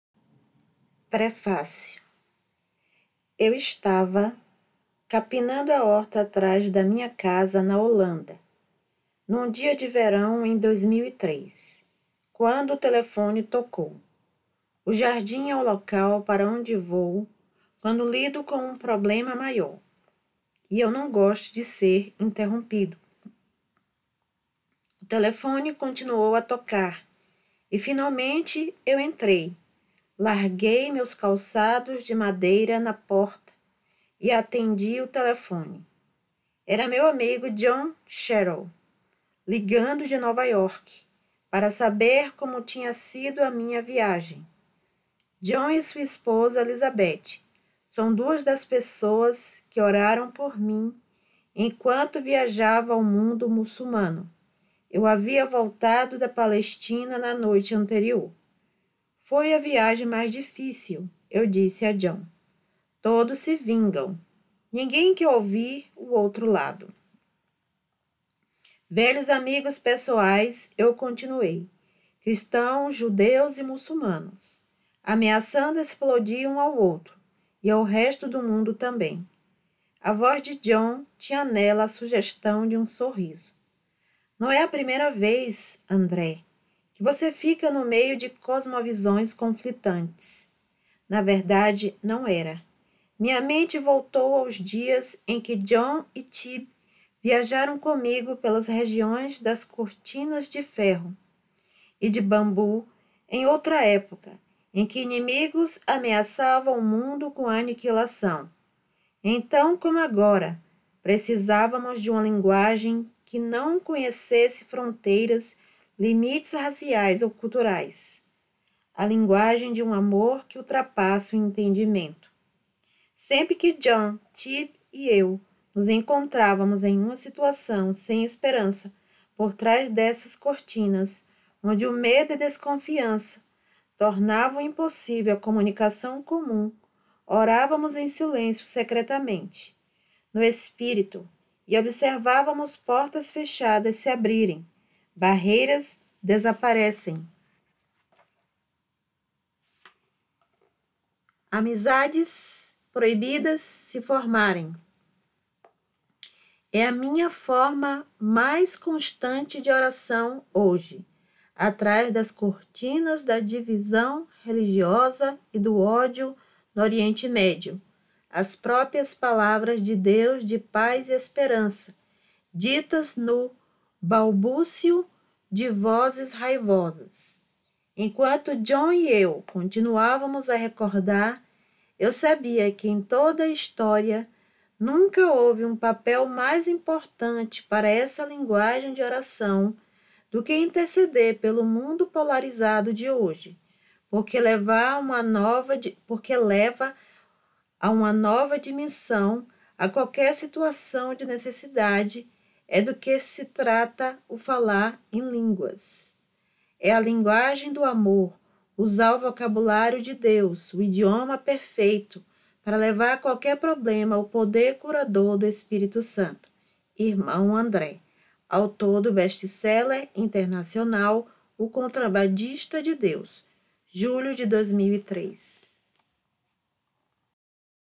05 ELES FALAM EM OUTRAS LÍNGUAS LIVRO JOHN SHERRILL PREFÁCIO
Livro sobre a realidade dos dons espirituais. Conta a história de um jornalista cristão cético quanto aos dons espirituais, e que após investigar constatou a veracidade e também passou pela experiência.